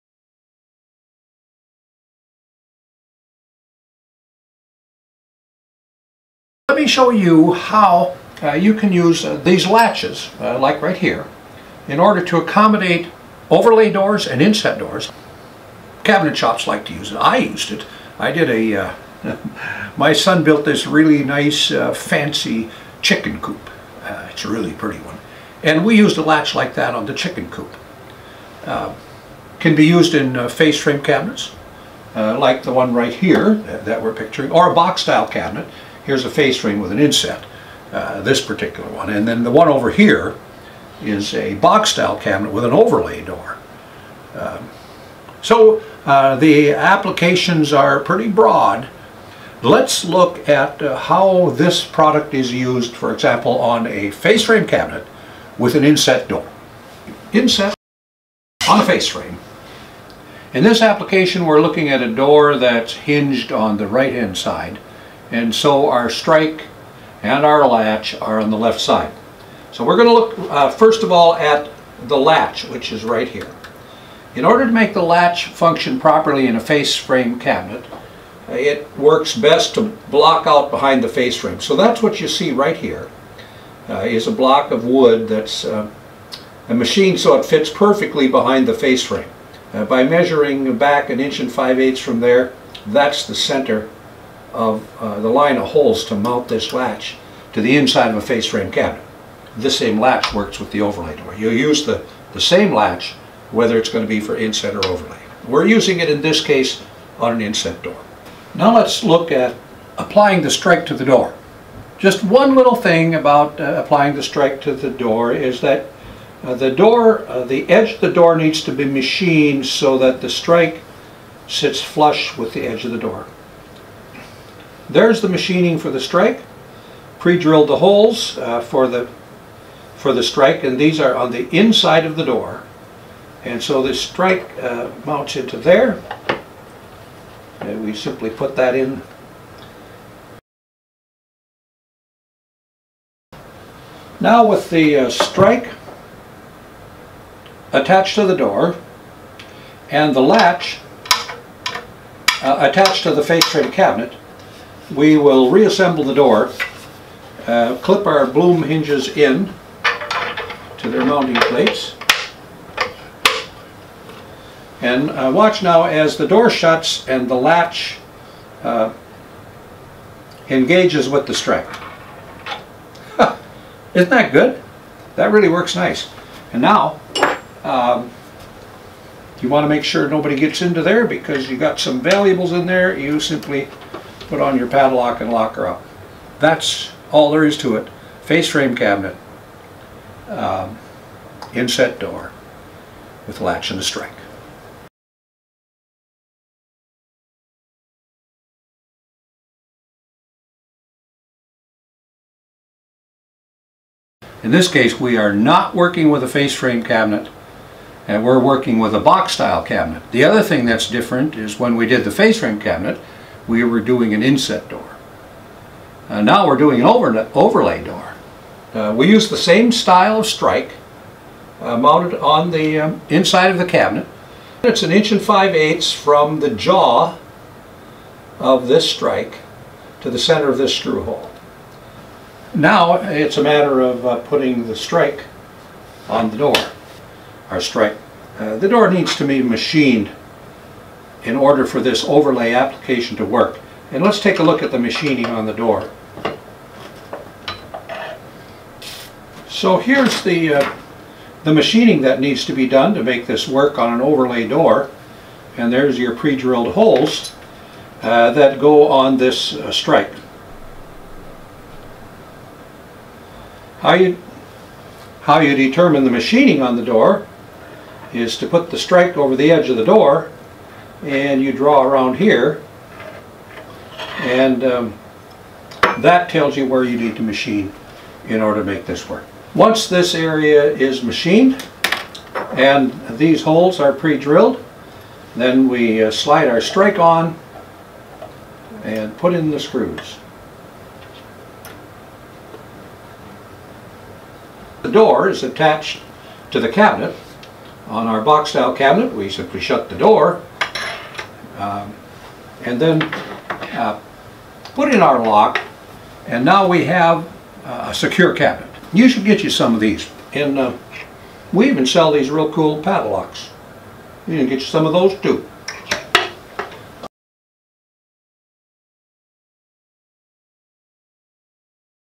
DIY Your Way - A Latch for Any Cabinet from RPC
RPC stay close locking and latching door hardware with a positive lock and the option to add a padlock. Can be used on Inset and overlay doors. For use on face frame and box style cabinets. Ideal for lockers in institutions and schools and storage cabinets for household cleaning items. Great for locking garage cabinets for tools and chemical storage. Applications for both commercial & residential.

Video 27